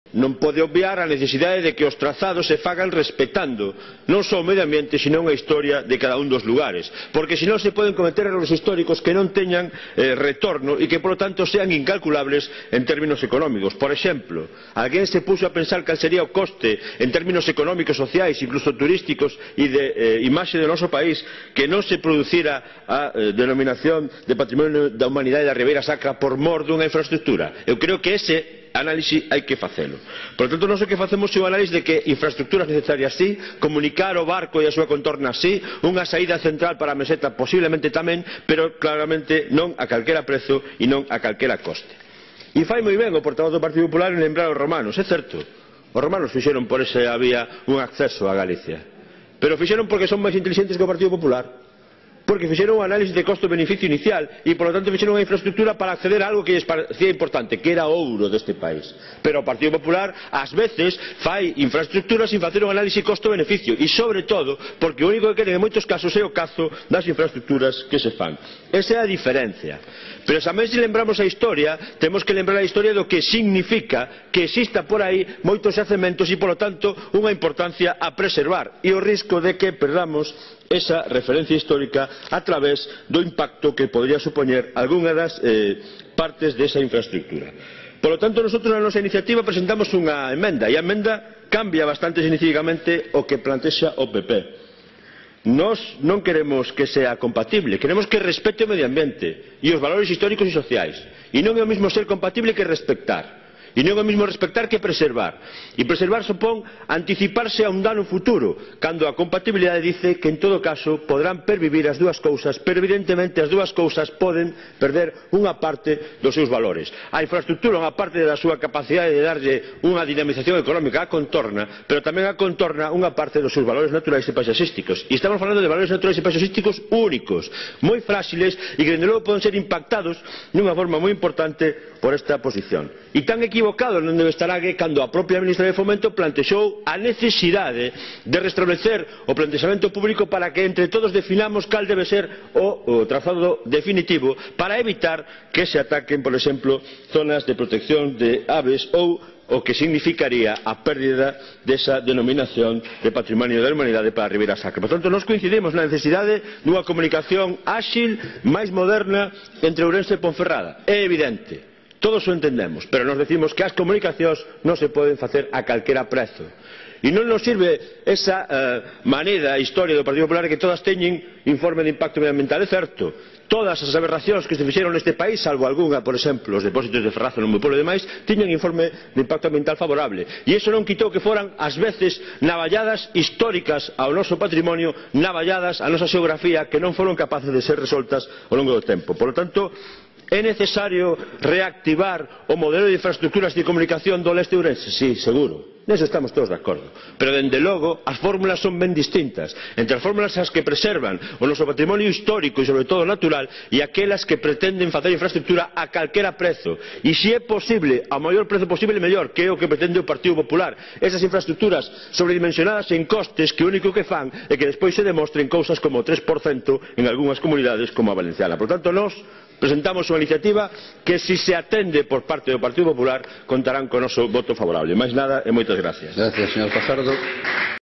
"Infraestruturas e comunicar o país si pero non a calquera custo"
O deputado de AGE, Juan Manuel Fajardo, salientou a importancia de analizar de xeito adecuado a relación entre o custo e o beneficio das infraestruturas repectando sempre o medio ambiente, os valores patrimoniais e paisaxísticos do noso país e o interese xeral e non só os intereses duns poucos e "poñer o cazo coa construción de infraestruturas"